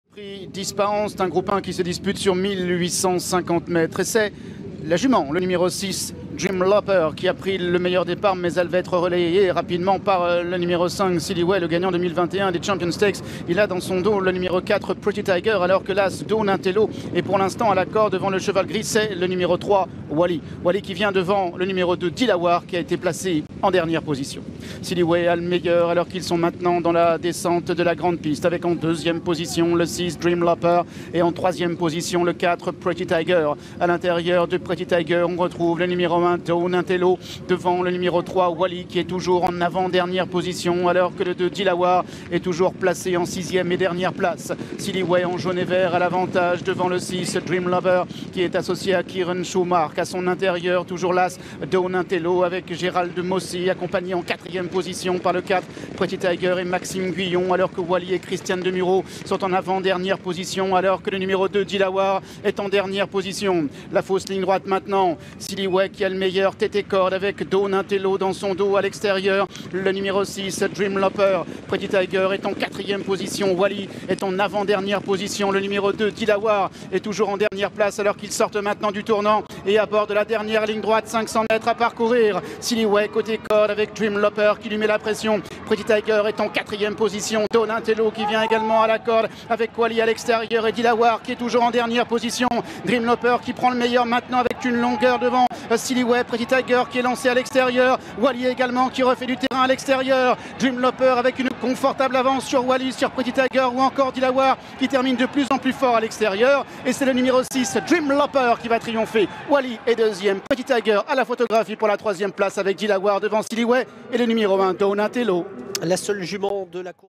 29 mai 2022, ParisLongchamp : Dreamloper bat les mâles dans l'Ispahan (Gr1)
Dimanche 29 mai 2022, Hippodrome ParisLongchamp (Paris). – Seule jument au départ du Prix d’Ispahan (Gr1), l’anglaise Dreamloper (Lope de Vega) venait de remporter les Dahlia Stakes (Gr2), à Newmarket, pour sa rentrée, devançant notamment la française Ebaiyra. Elle avait conclu l’exercice 2021 par une troisième place dans les Sun Chariot Stakes (Gr1). Dimanche, elle a créé la surprise en décrochant son premier Groupe 1 sur les 1 850 mètres de ParisLongchamp. Il fallait remonter à Golden Lilac (Galileo), en 2012, pour retrouver trace d’une femelle au palmarès de cette épreuve, qui faisait suite au doublé de la grande Goldikova (Anabaa). La pensionnaire d’Ed Walker a dominé de deux longueurs le bon finisseur Wally (Siyouni). Très en vue, Pretty Tiger (Sea The Moon), Dilawar (Dubawi) et Sealiway (Galiway) ont dû se contenter des places.

La course a été rapidement emmenée par Sealiway, suivi par Dreamloper. Dans la ligne droite, la partenaire de Kieran Shoemark a progressivement pris la mesure de ses rivaux, qui se sont disputés les accessits.

Elevée par Olivia Hore, la lauréate est une fille de Lope de Vega, auteur du doublé Poule d’Essai-Prix du Jockey Club, en 2010. Sa mère Livia’s Dream (Teofilo) s’est placée au niveau listed en Angleterre. Son frère hongre a été vendu 27 000 £ en fin d'année dernière à Tattersalls. La famille remonte à la bonne Yawl (Rainbow Quest), mère de Prowess (Peintre Célèbre), 3ème des Cheshire Oaks (L) à Chester, qui finit sa carrière par un échec au Croisé-Laroche sous l'entraînement de Richard Gibson, mais produisit la gagnante des Oaks 2013 Talent (New Approach), d'où la talentueuse Ambition (Dubawi), double lauréate du Prix Fille de l'Air (Gr3) et placée du Prix Jean Romanet (Gr1).